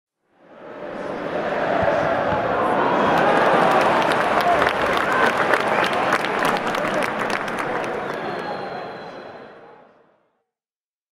Stadium Crowd - Sound Effect (Free No Copyright Sounds)
Stadium Crowd sound effect by Bunksound